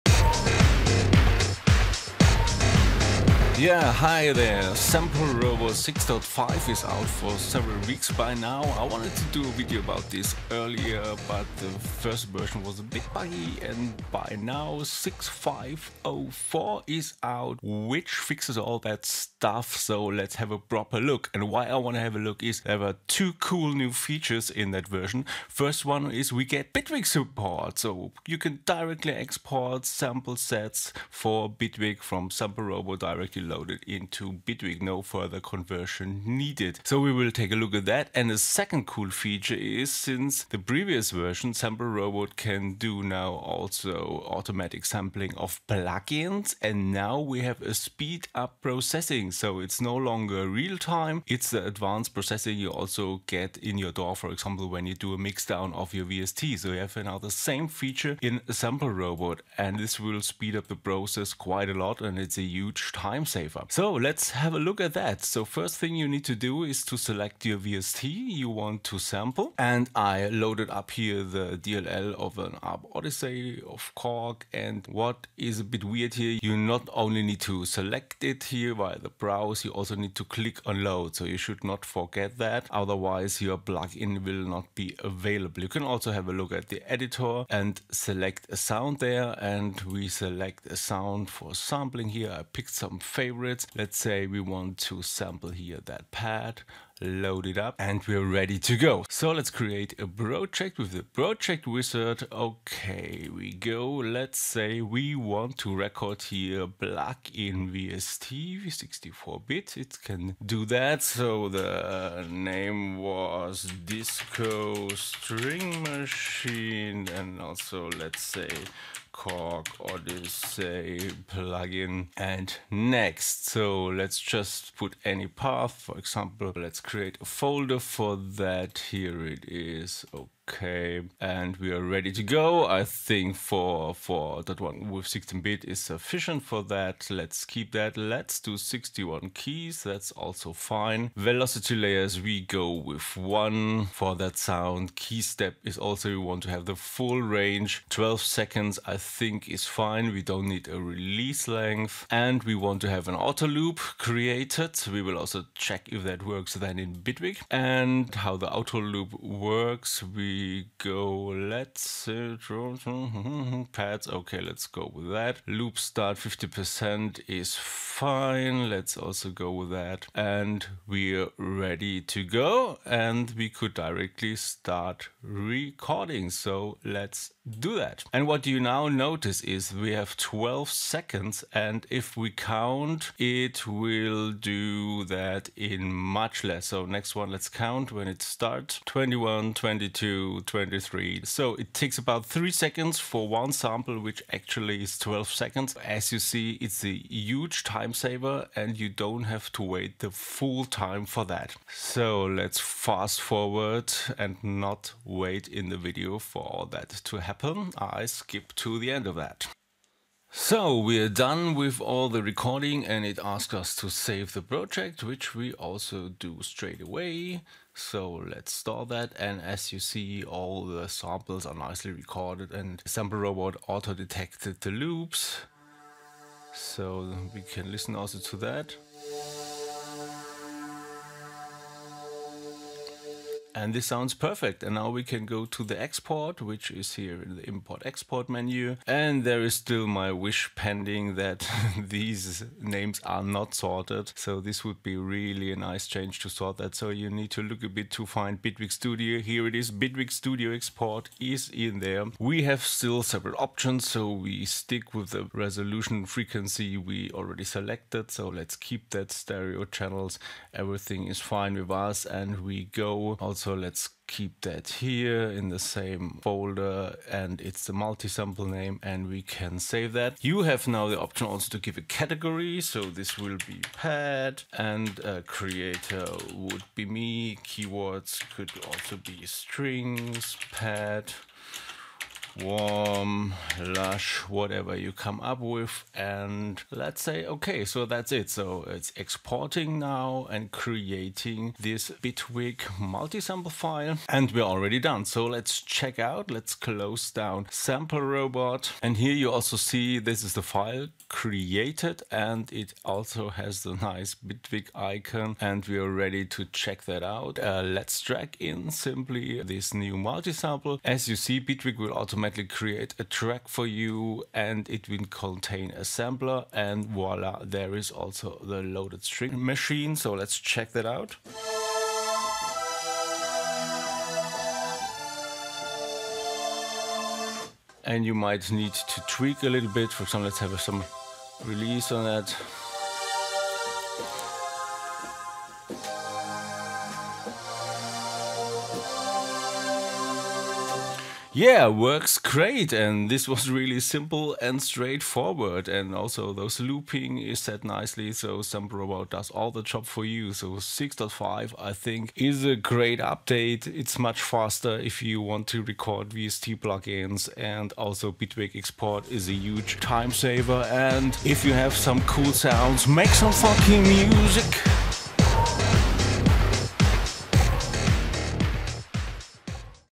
SampleRobot 6.5 - Bitwig Support and VST offline bouncing!
New SampleRobot version is out and we can now directly export to Bitwig Studio! Furthermore, if you sample a sound from a VST the process is no longer executed in realtime, instead the much faster offline bouncing is applied.